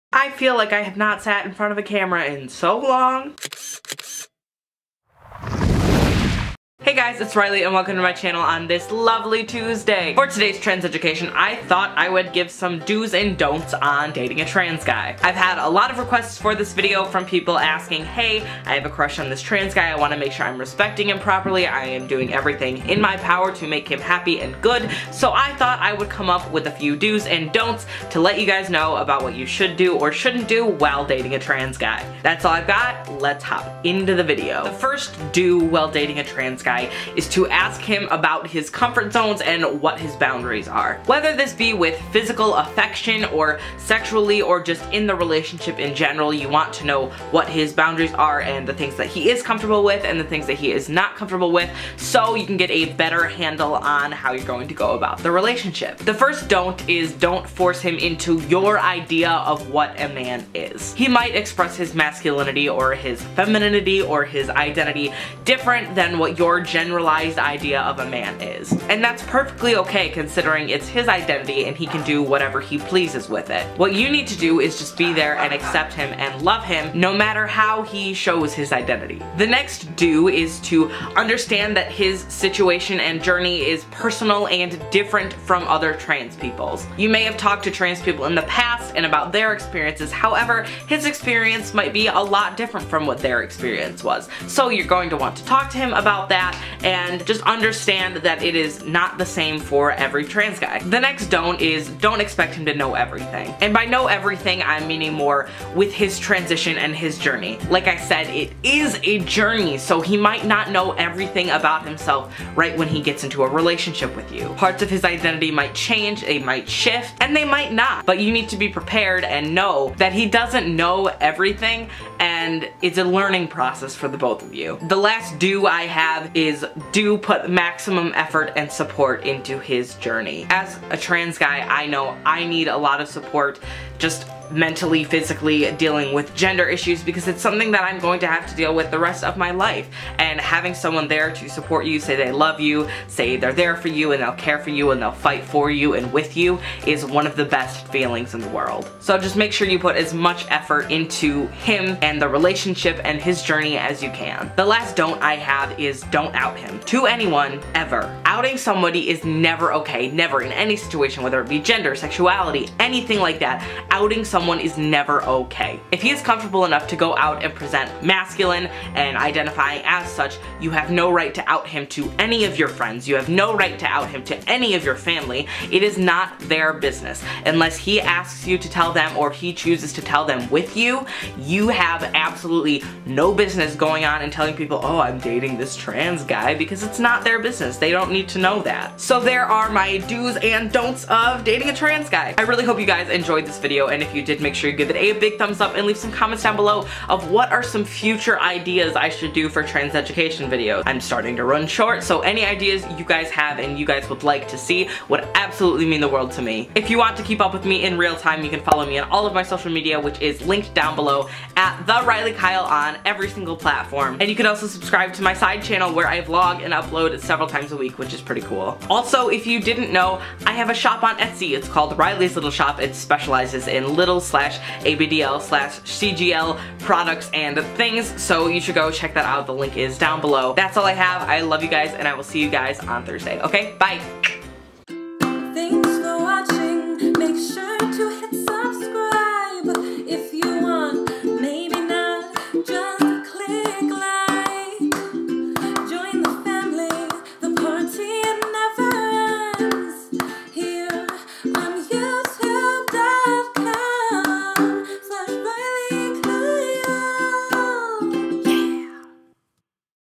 DATING A TRANS GUY //
☆ Some do's and dont's for dating a transguy.